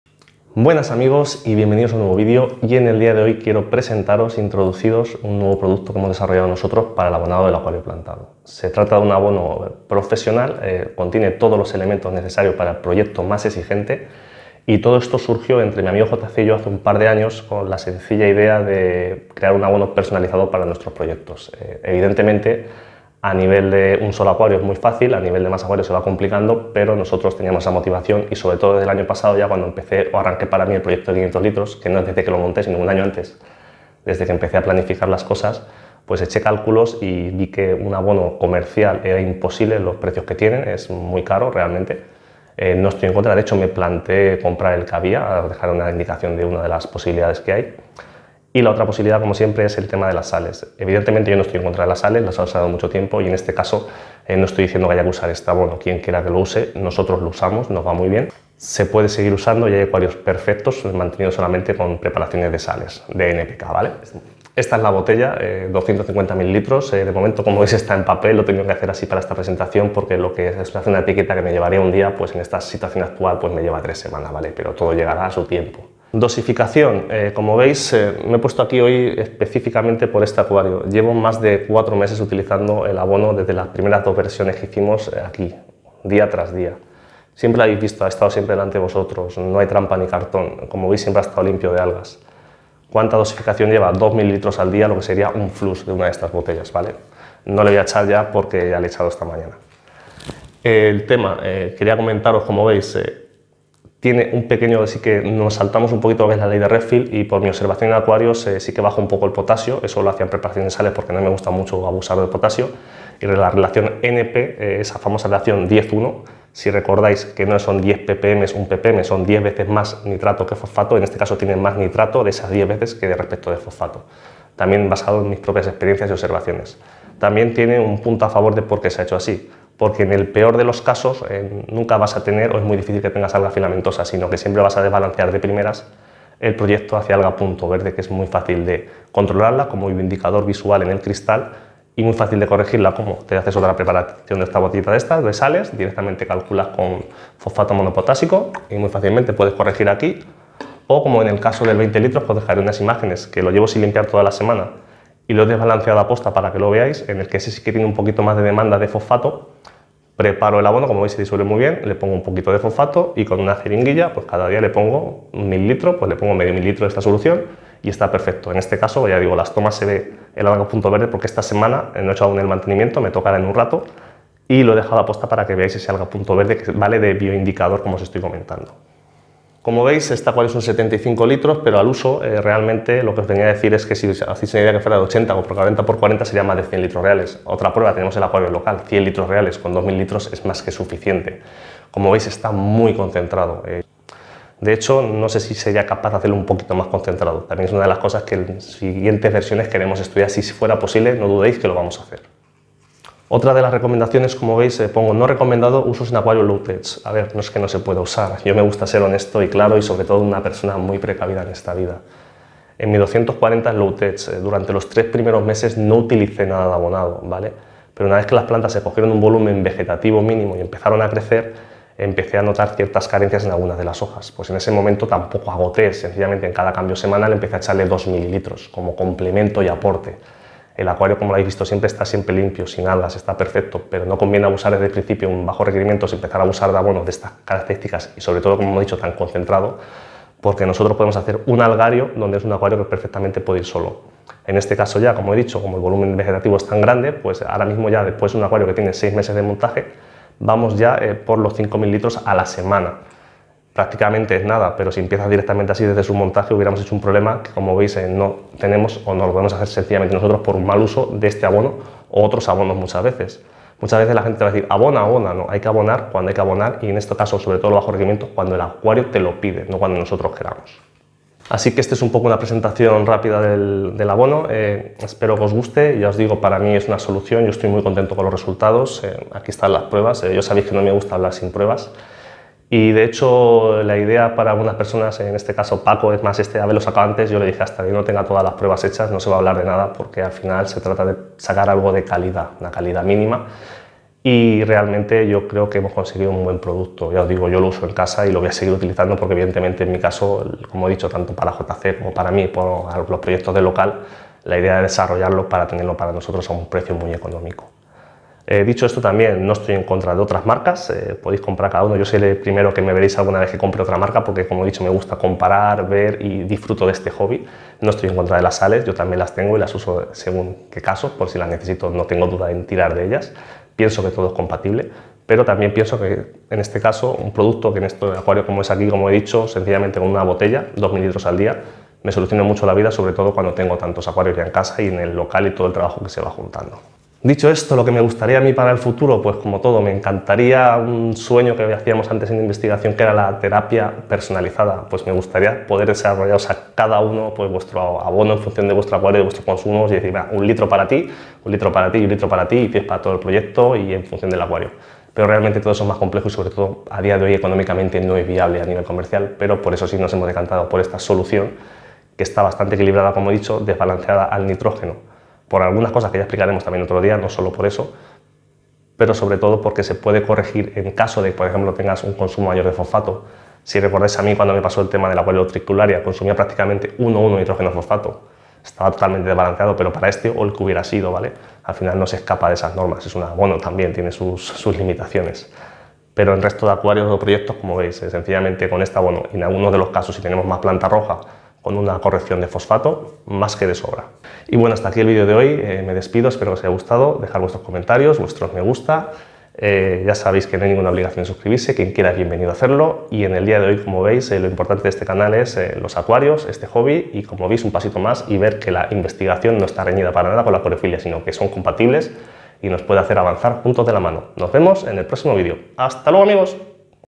Cómo Abonar los Acuarios Plantados #4: Fabricamos nuestro Abono Personal Completo NP4K
En el día de hoy os dejo un vídeo sobre la historia y origen de este abono, que es muy personal y especial para mi. No ha sido algo fácil, puesto que implica trabajo y medidas de tasas de consumo durante mucho tiempo. Pero tras varios meses esta listo para usarlo en mis otros proyectos de acuario plantado de altos requerimientos (especialmente en mi 500 litros)